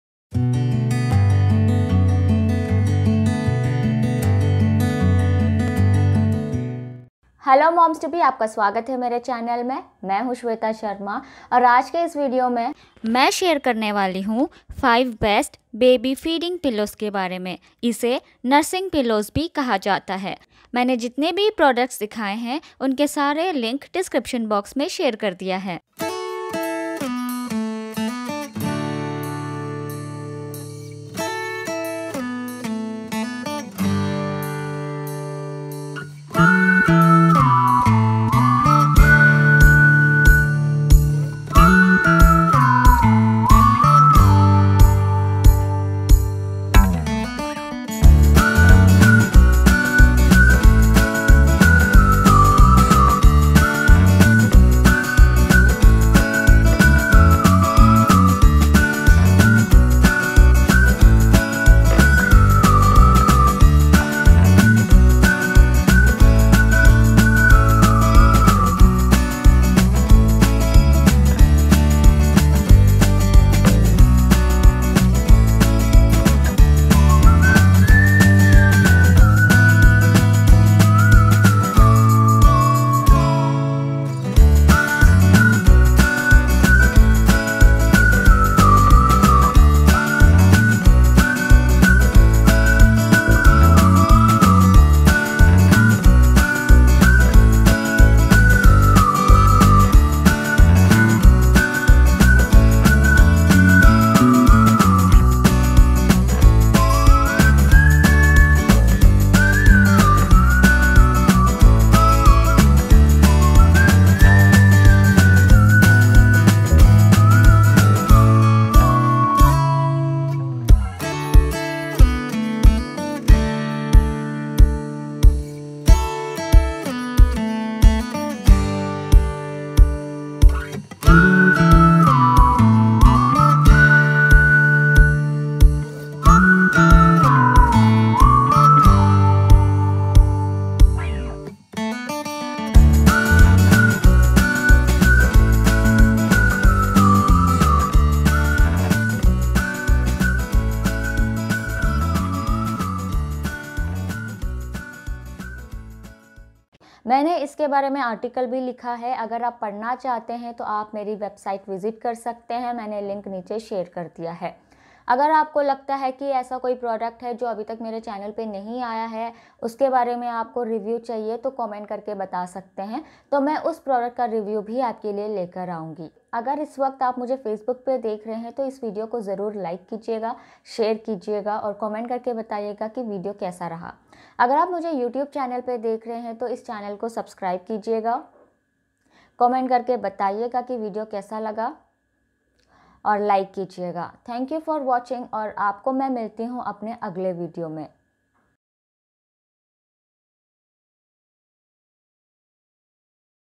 Baby Feeding Pillow | 5 Best Nursing Pillows | Feeding Pillow Breastfeeding Mother
Feeding baby pillows are designed in different shapes and styles like Crescent Roll, O-Shaped, C-Shaped pillow, curved Shaped, etc. It's helpful for new mothers while recovering from C-section and provides easy feeding for babies.

Is Feeding/ Nursing Pillow that important?
Yes, it helps to hold the body of the baby while feeding securely. Support newborn baby’s head, neck, back, and body while feeding.

It's helpful for new mothers while recovering from C-section and provides easy feeding for babies.

Baybee Elevato Breast Feeding Pillow
Kradyl Kroft
Chicco Boppy Pillow with Slipcover
Baybee New Breast Feeding Pillow
Nina Baby Feeding Pillow